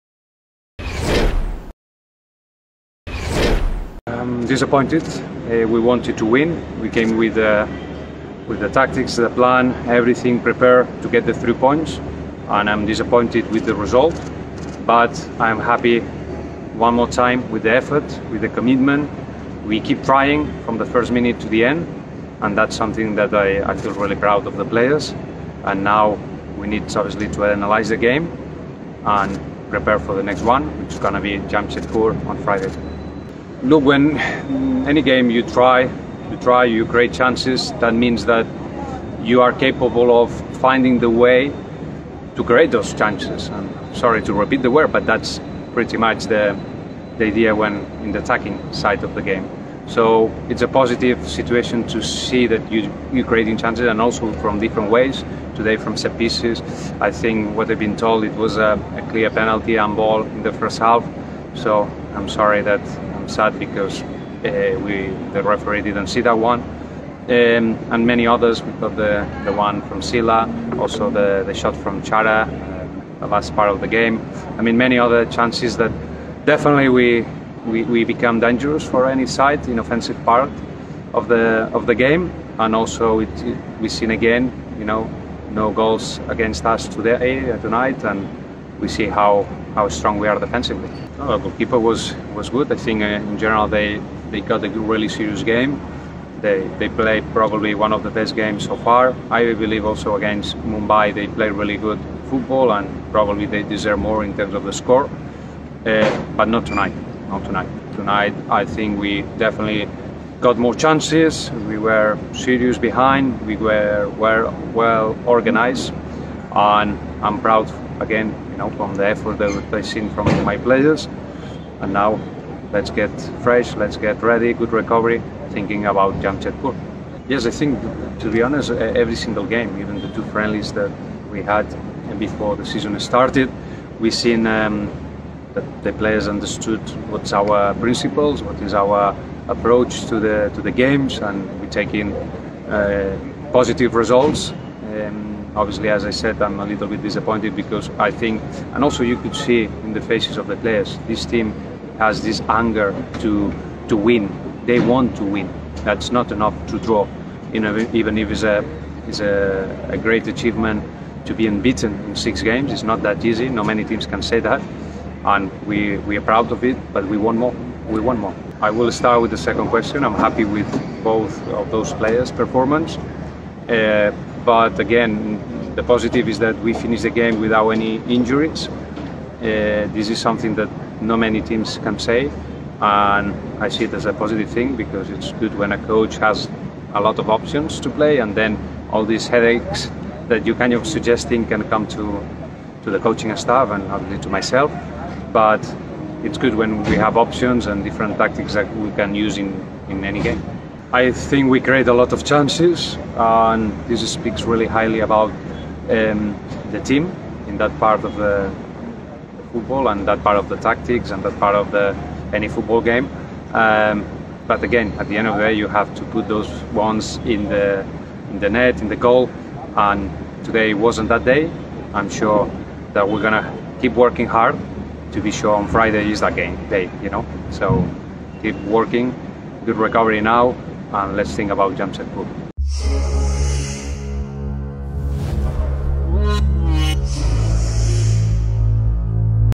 Post Match Media Interaction NEUFC vs Chennaiyin, Hero ISL 7
Coach Gerard Nus reflects on the draw against Chennaiyin FC.